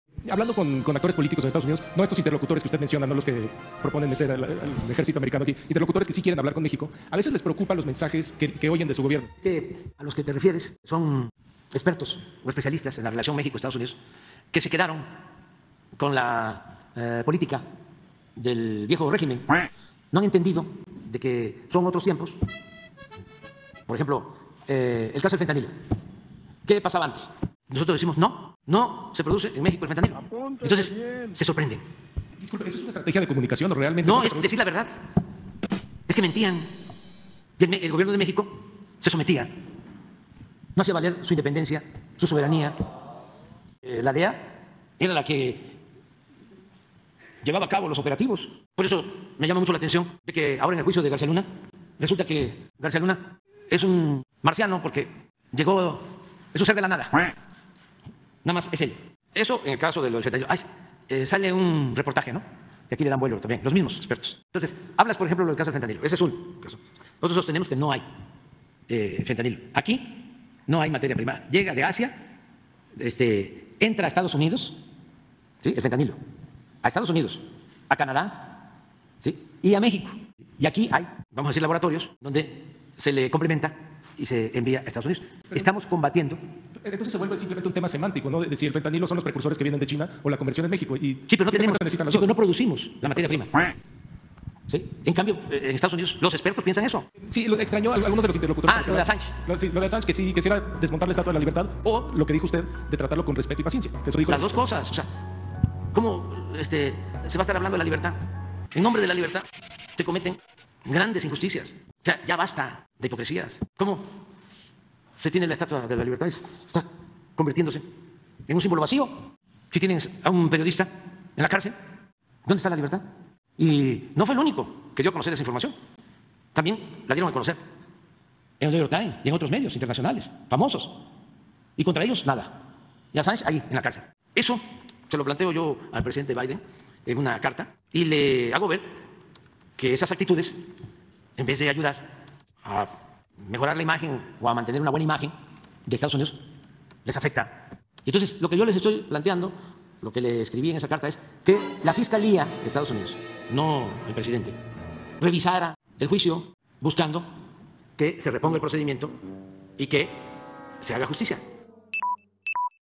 México no produce Fentanilo, e insiste que EEUU 🇺🇸 debe reconsiderar la detención de Assange 🦆
Reitera que México no produce Fentanilo, e insiste que el Gobierno de Estados Unidos 🇺🇸, debe reconsiderar la detención de Julian Assange 🦆,